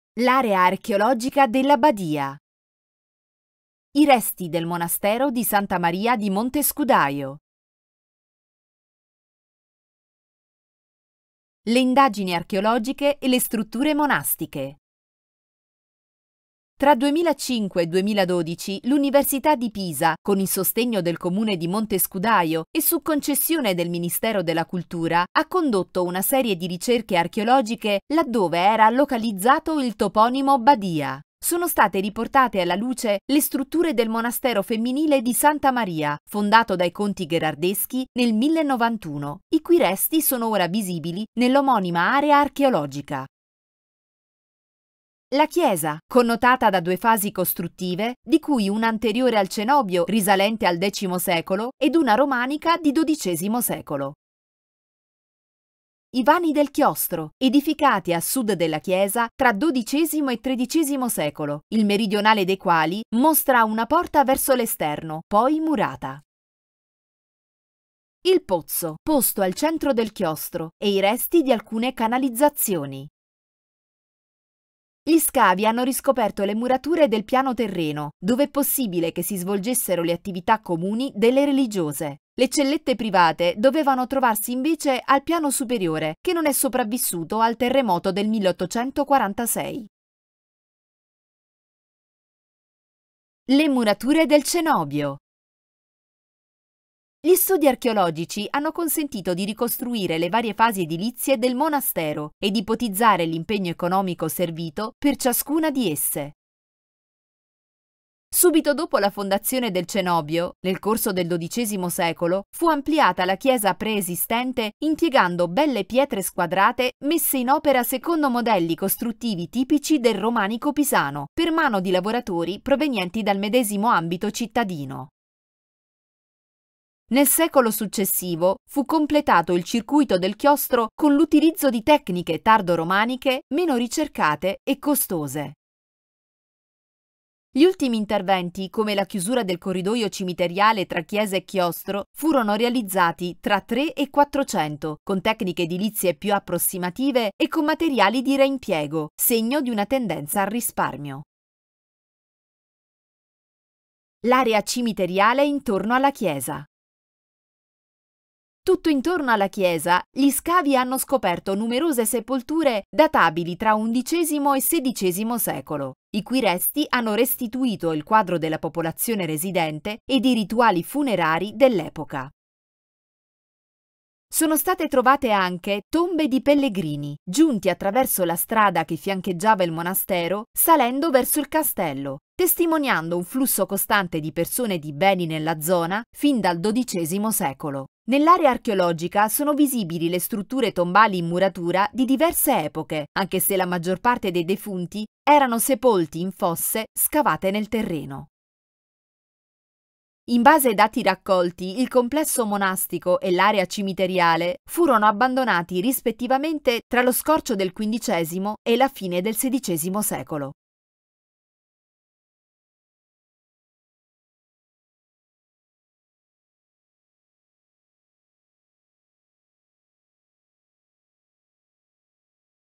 Teca10 - L’area archeologica della Badia
00:05 - I resti del monastero di S. Maria di Montescudaio
00:12 - Le indagini archeologiche e le strutture monastiche
01:33 - Le murature del cenobio
02:35 - L’area cimiteriale intorno alla chiesa